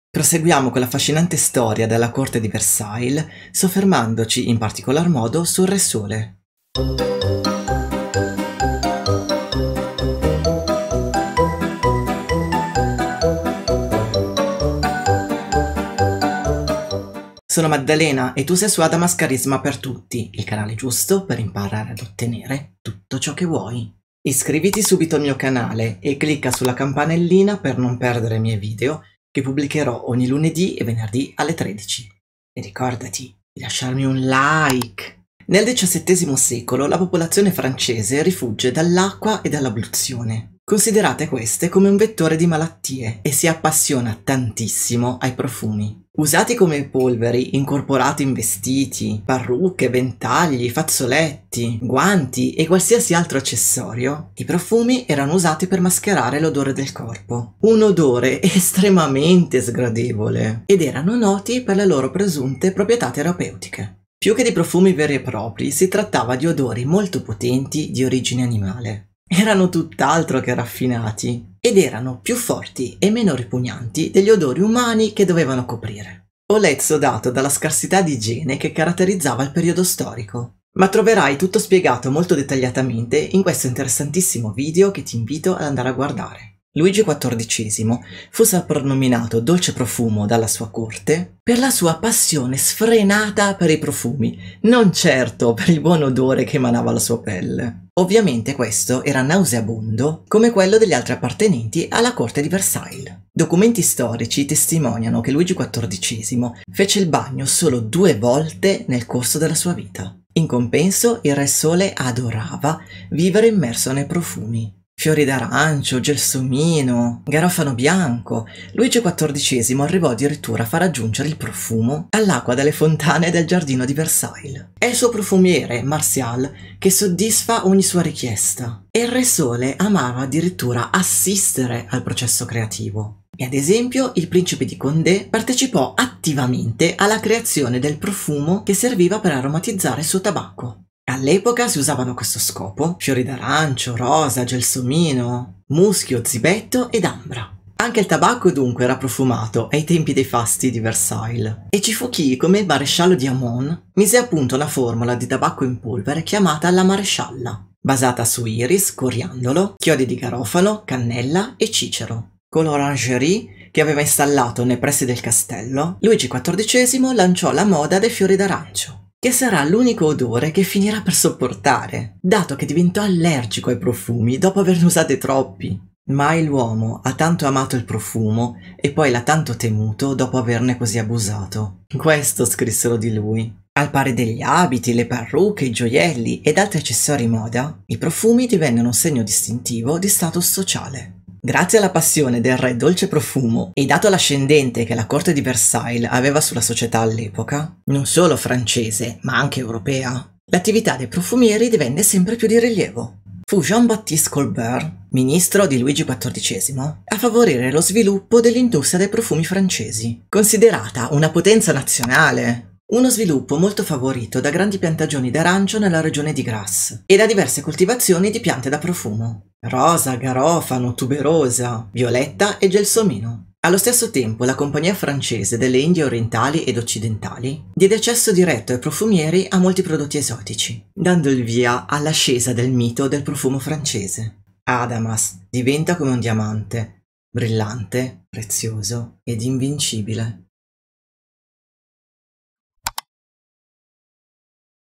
I profumi del RE SOLE 💎
Proseguiamo con l'affascinante storia della corte di Versailles soffermandoci in particolar modo sul Re Sole.